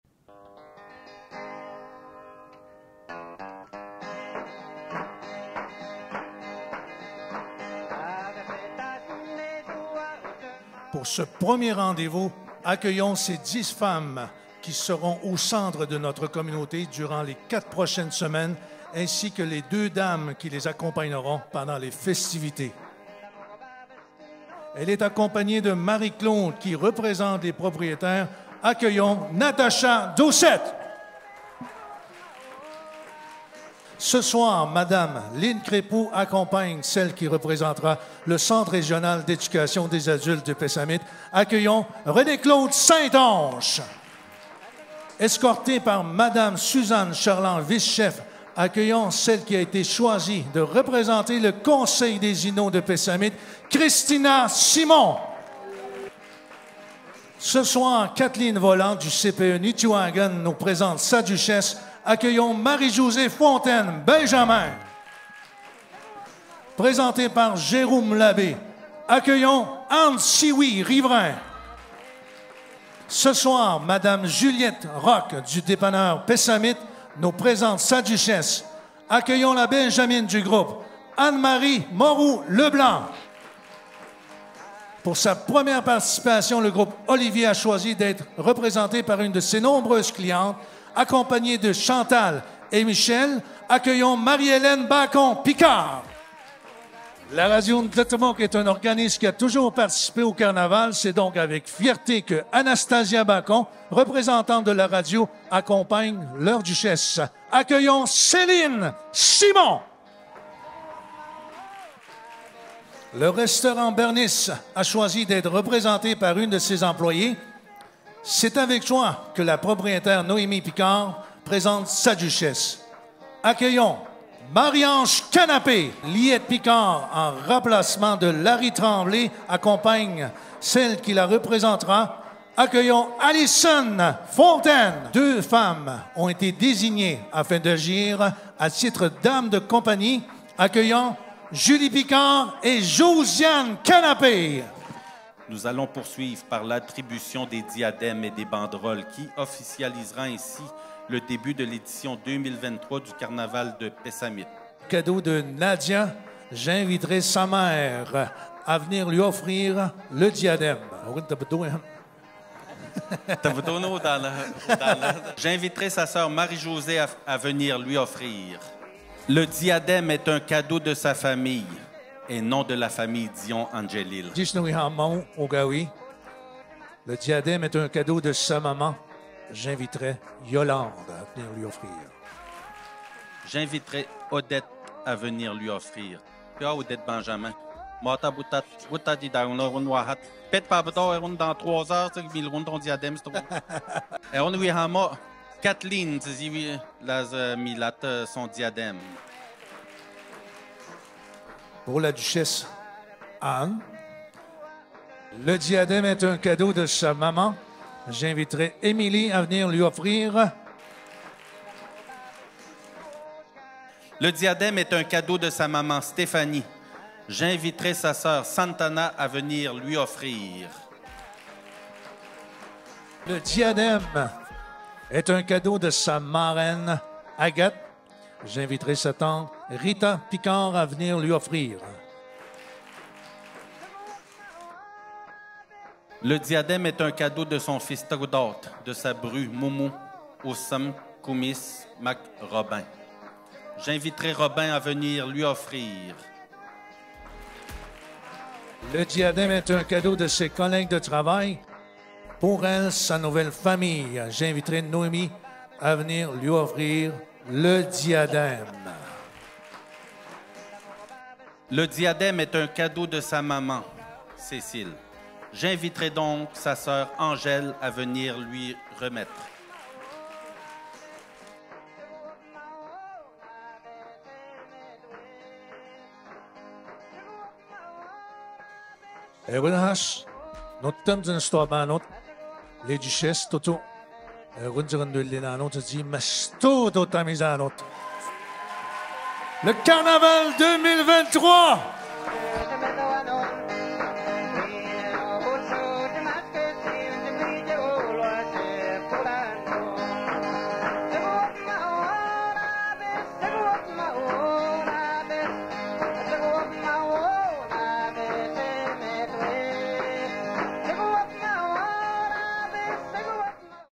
Carnaval de Pessamit 2023 - Présentation des duchesses
Vidéo récapitulatif de la première soirée du Carnaval de Pessamit : La présentation des duchesses